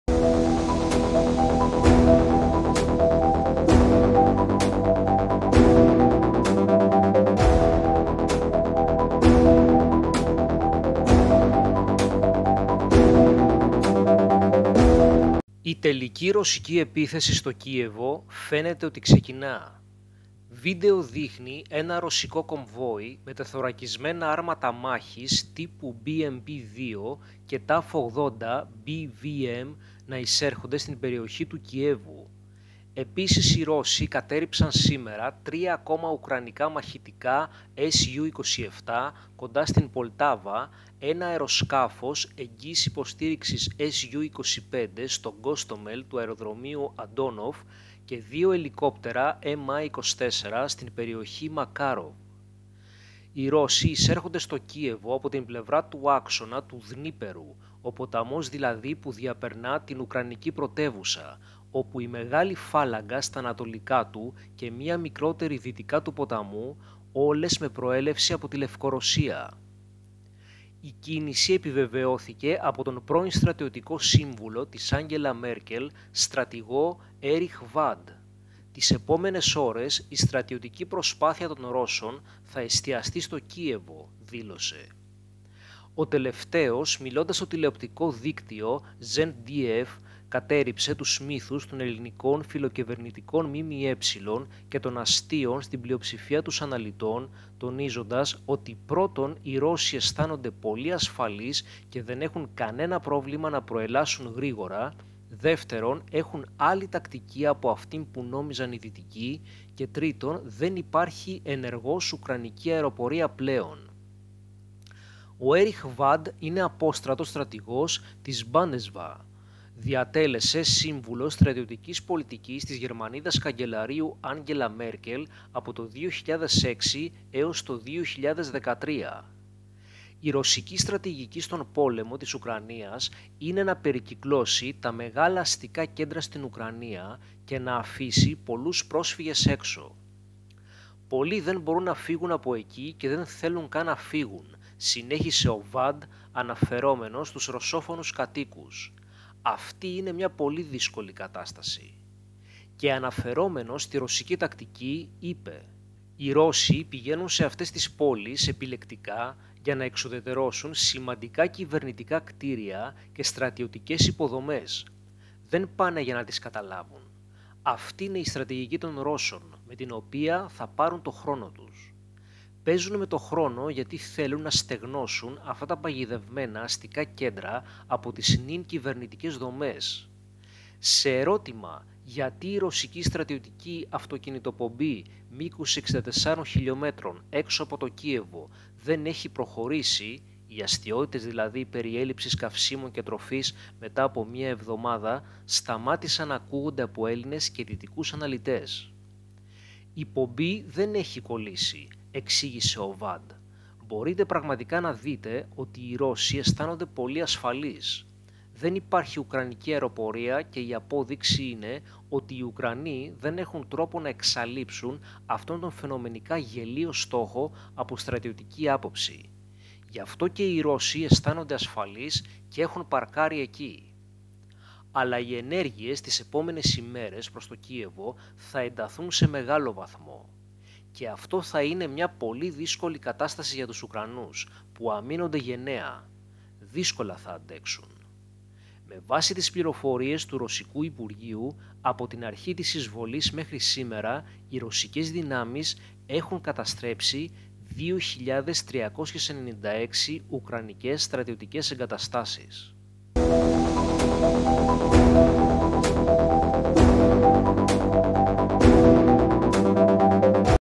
EKTAKTO: Η τελική Ρωσική επίθεση στο Κίεβο ξεκινάει!! Κάποια πράγματα όμως...σας τα διαστρέβλωσαν!!
ΠΗΓΗ: PRONEWS